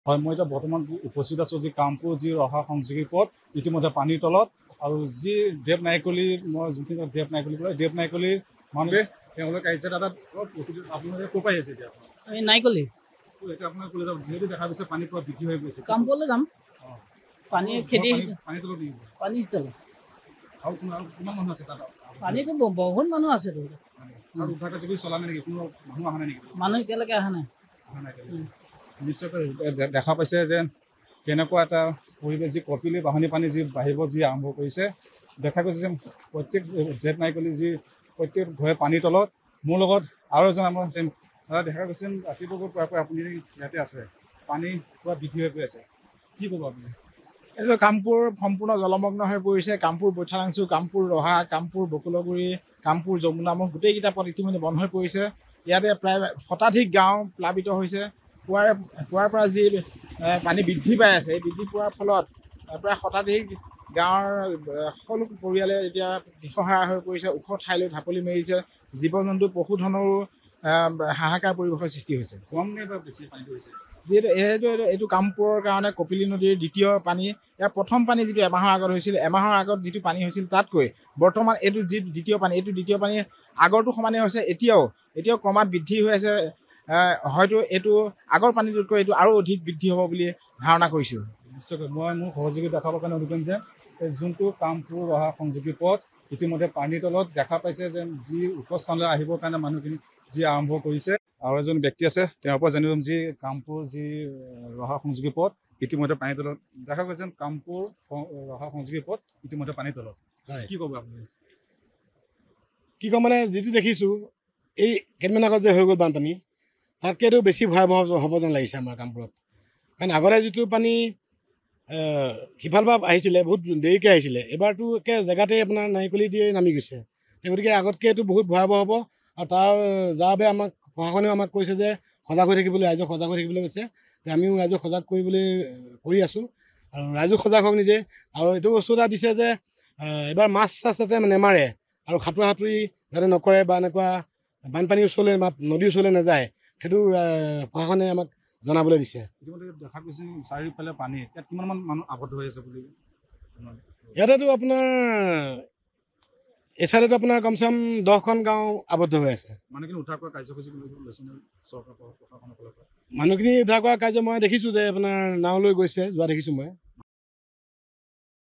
কামপুৰত কপিলী নৈৰ কালৰূপ
কপিলীৰ বানত জলমগ্ন অৰ্ধশতাধিক গাঁও